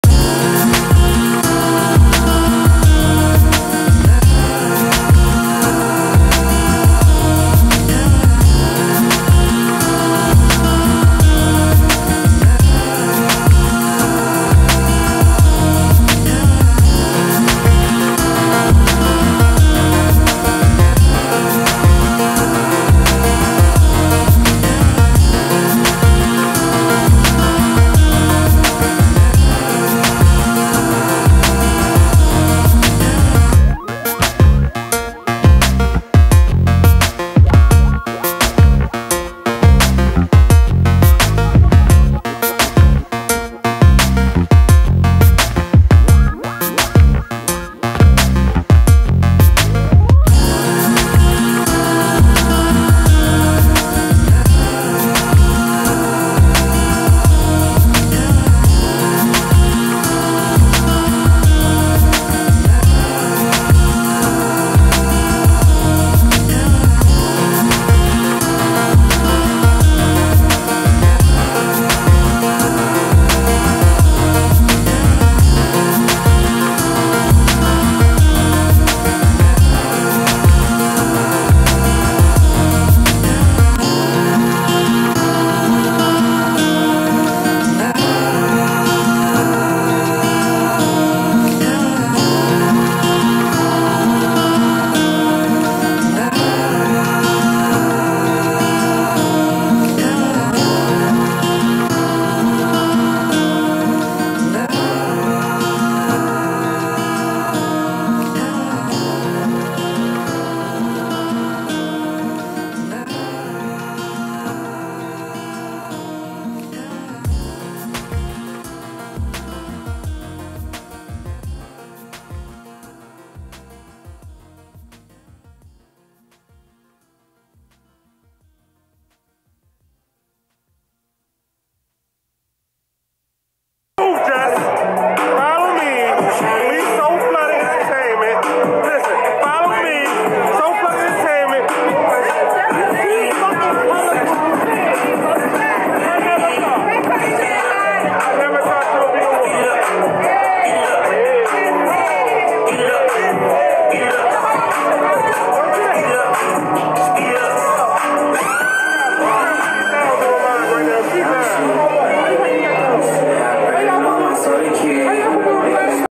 Jess hilarious partying! 😁
Jess is out at the pub, dancing twerking -knowing she all dat! Lol,...but she really is